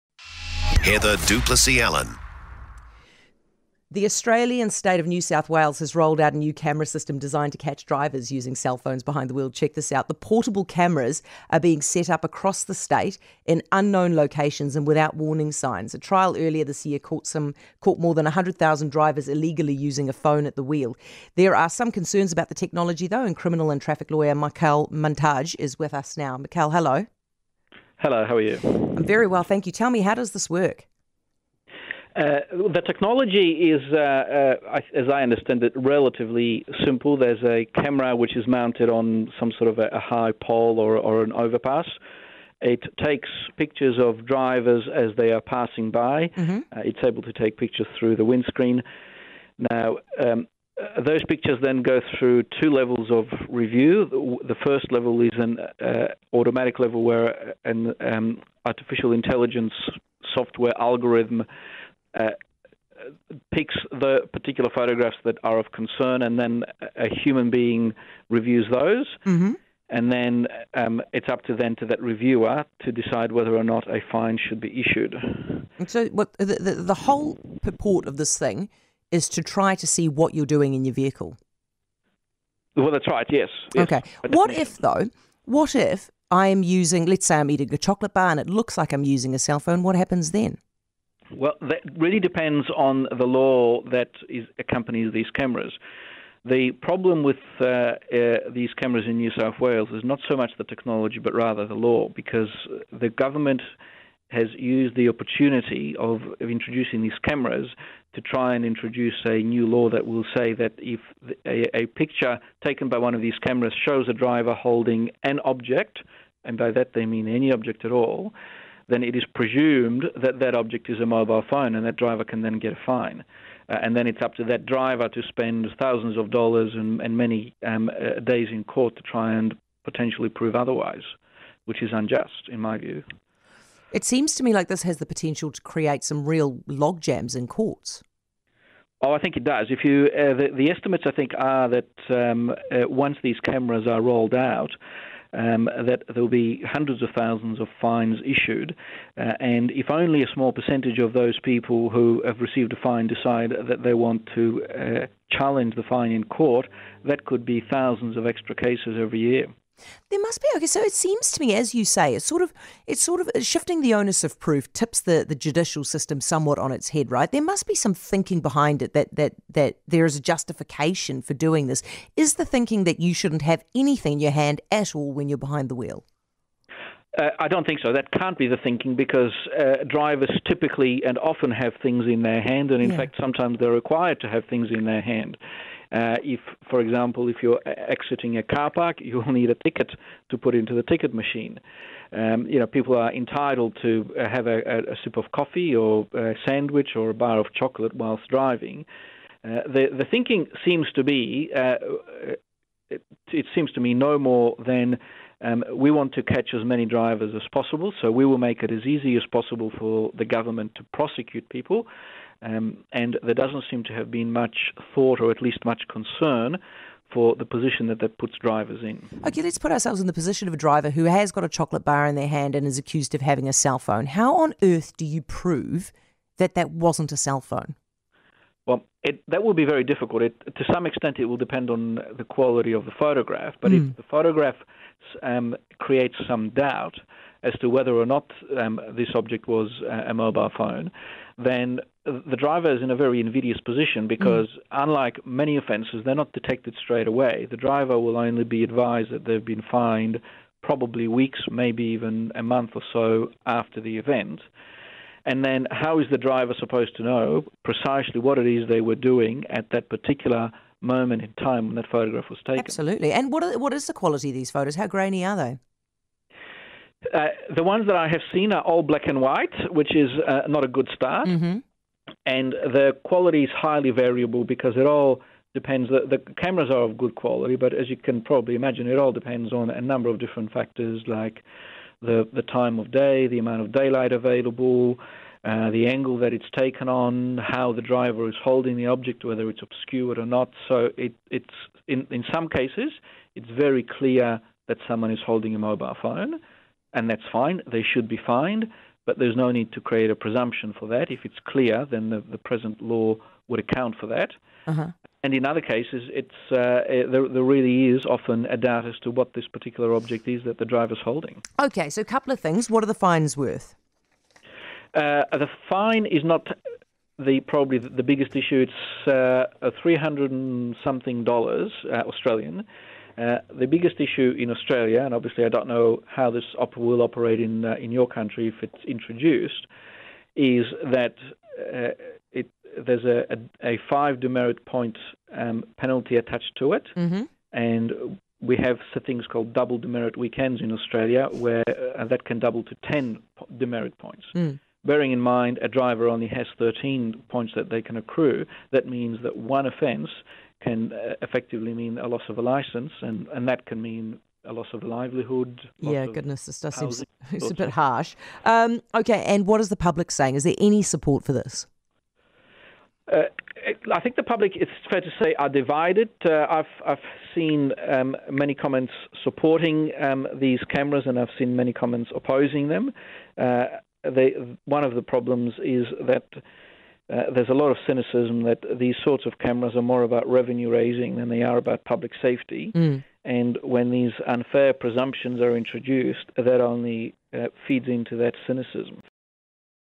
Michal Mantaj Remarks on Phone Detection Cameras - NewsTalk ZB
Michal Mantaj remarks on mobile phone detection cameras during an interview with NewsTalk ZB.

Michal Mantaj is a Trial Advocate, Accredited Criminal Law Specialist and Director at Conditsis Lawyers.

Mobile phone detection cameras started targeting phone use across NSW on December 1st. The system uses artificial intelligence to automatically review images and detect offending drivers.